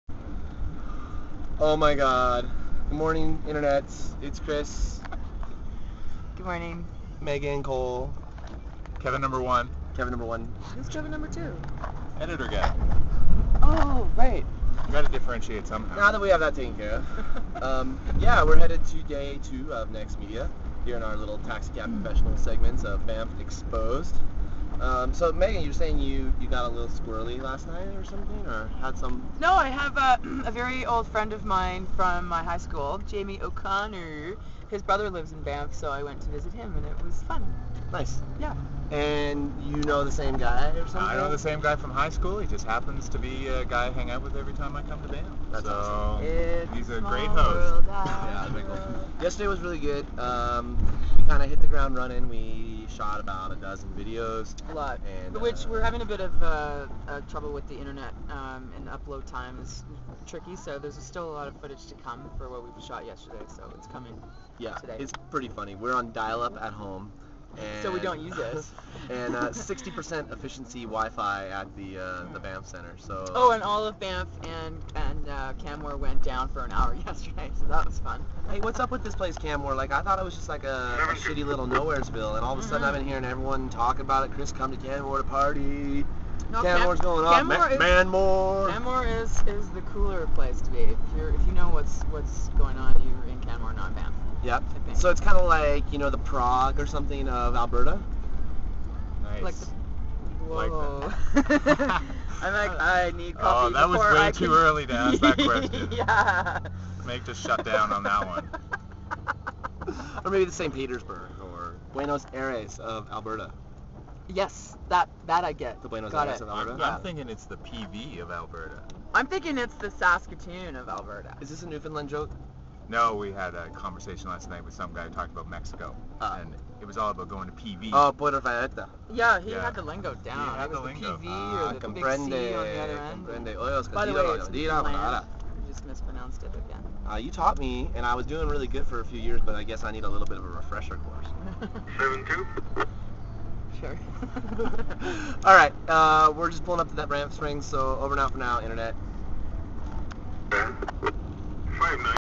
Banff Exposed - Taxi Cab Confessions Day 2 - nextMEDIA 2008
MC, kk, and Kevin rollin' to the nextMEDIA conference in Banff on Day. Good morning internets!